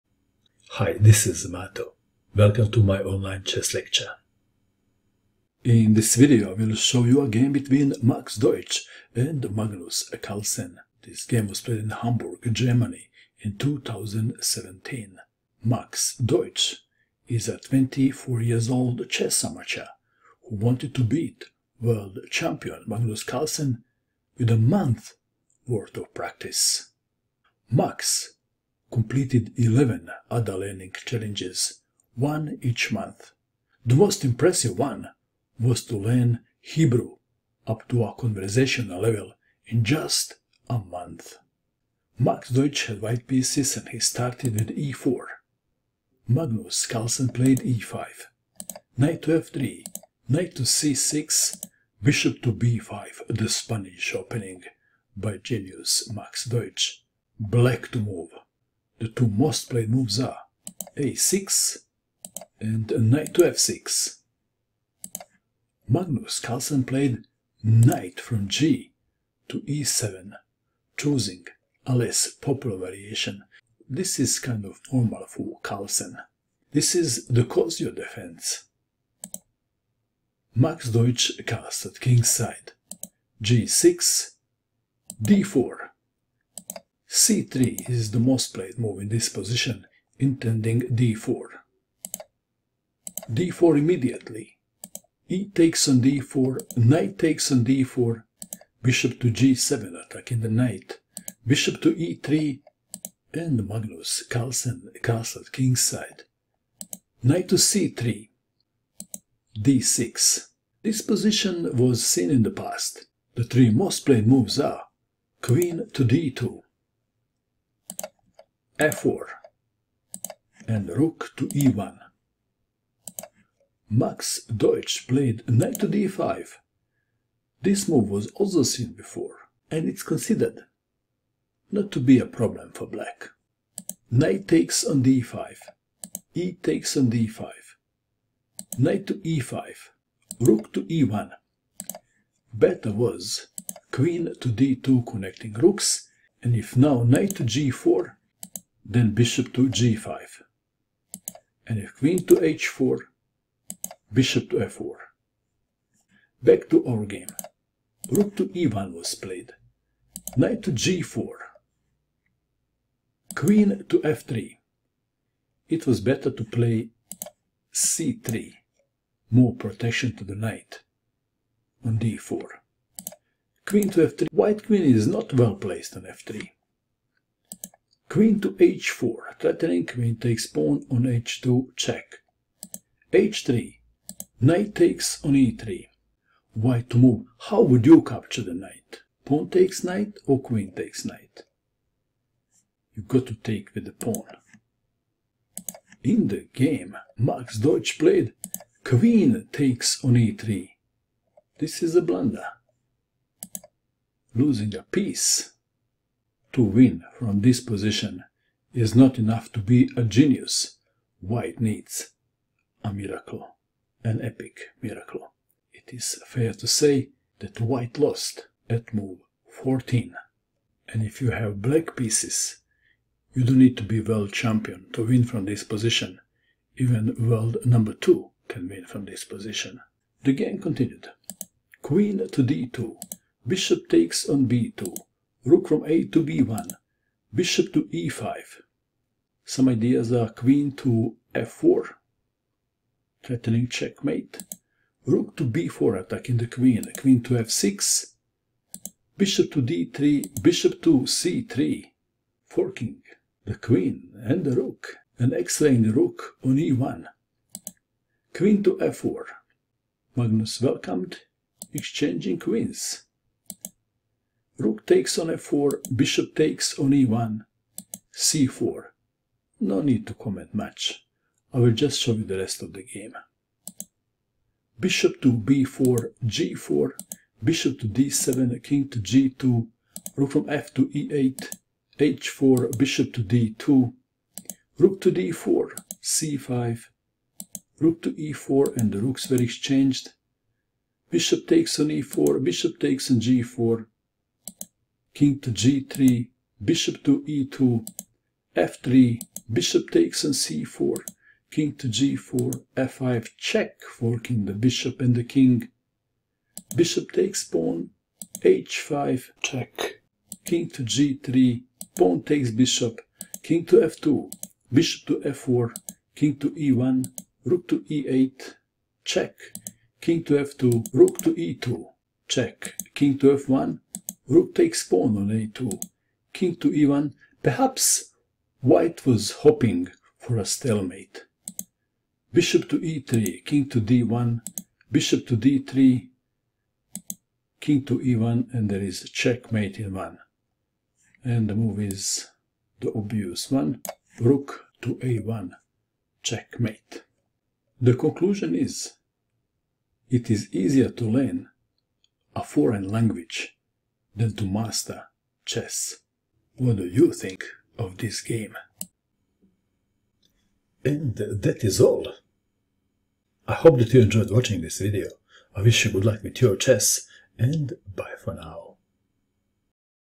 Max Deutsch vs Magnus Carlsen: Hamburg 2017
Genius vs World Champion
Max Deutsch vs Magnus Carlsen
Exhibition Game Hamburg GER 2017
Spanish Game: Cozio Defense. General (C60)
1. e4 e5 2. Nf3 Nc6 3. Bb5 Nge7 4. O-O g6 5. d4 ed4 6. Nd4 Bg7 7. Be3 O-O 8. Nc3 d6 9. Nd5 Nd5 10. ed5 Ne5 11. Re1 Ng4 12. Qf3 Qh4 13. h3 Ne3 14. Qe3 Bd4 15. Qd2 Bb2 16. Rab1 Be5 17. Rb4 Qf6 18. Bd3 Bc3 19. Qf4 Qf4 20. Rf4 Be1 21. c4 Bb4 22. g4 Bd7 23. Kg2 Rfe8 24. h4 Bd2 25. Rd4 c5 26. Re4 Re4 27. Be4 Bg4 28. Kg3 Be2 29. f3 Bc4 30. Kg4 f5 31. Bf5 h5 32. Kg3 gf5 33. Kf2 Bf4 34. Ke1 Re8 35. Kf2 Re2 36. Kf1 Ra2 37. Ke1 Be3 38. Kd1 Bd3 39. Ke1 Ra1#
 0-1